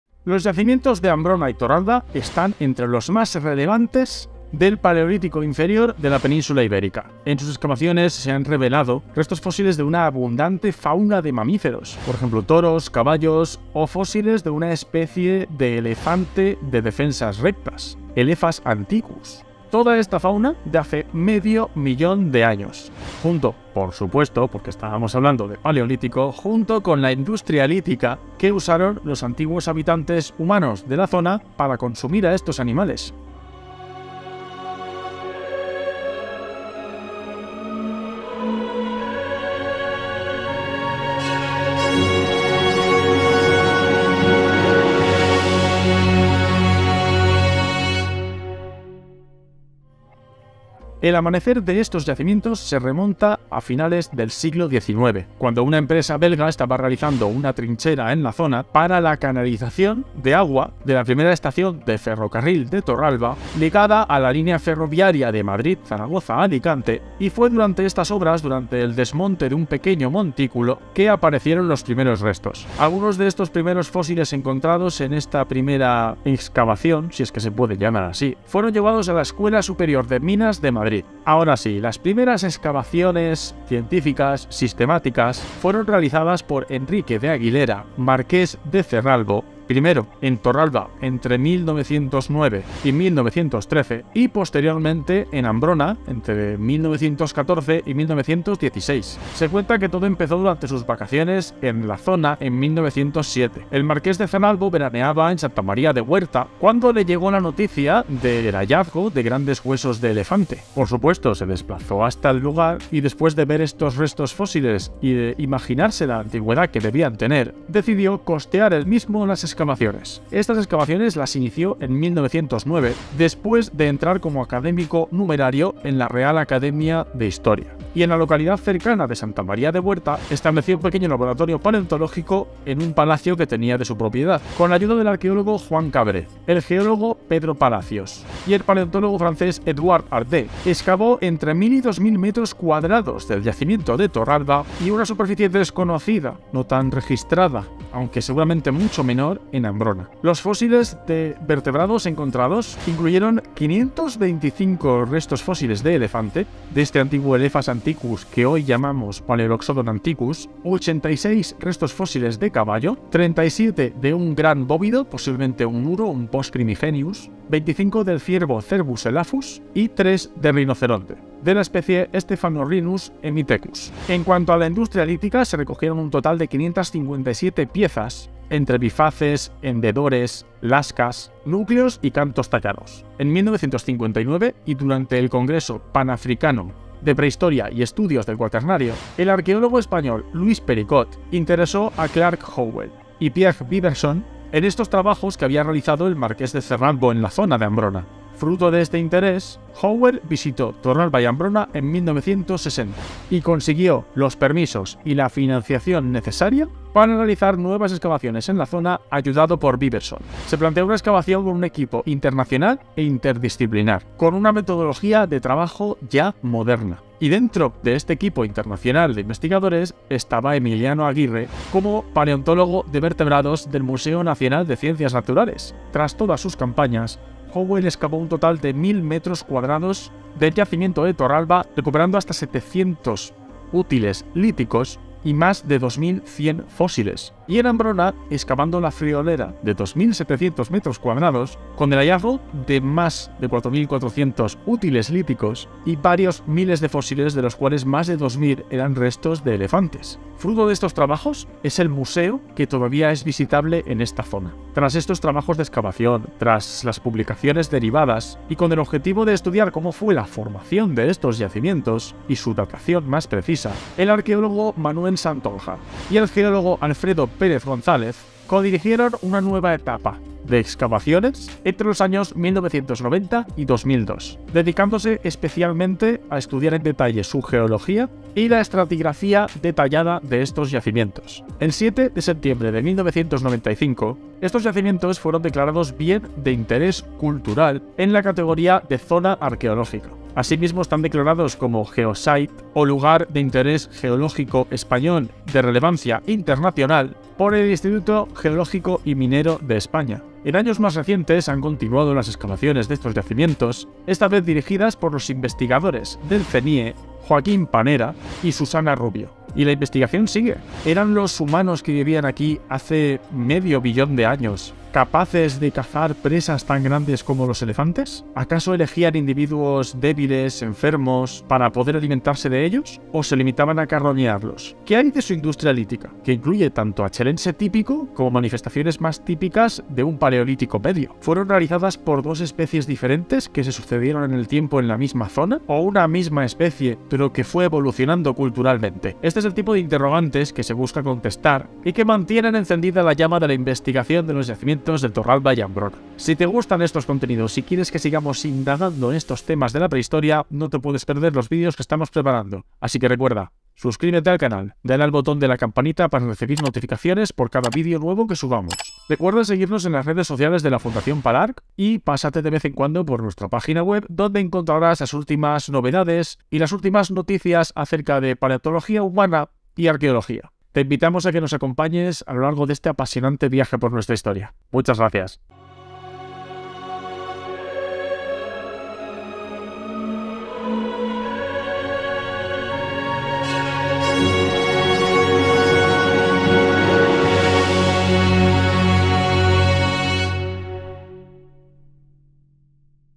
Los secretos del Paleolítico Inferior en la Península Ibérica | Yacimientos de Ambrona y Torralba
En este vídeo Francesc Gascó nos habla sobre los yacimientos de Ambrona y Torralba, que son importantes en la Península Ibérica por su abundante fauna de mamíferos del Paleolítico Inferior, así como la industria lítica utilizada por los antiguos habitantes humanos. Se describe la historia de las excavaciones en ambos sitios, desde el descubrimiento inicial en el siglo XIX hasta la investigación actual dirigida por Joaquín Panera y Susana Rubio. También se discuten las preguntas sin respuesta sobre la capacidad de los humanos para cazar presas tan grandes y la evolución cultural de la especie.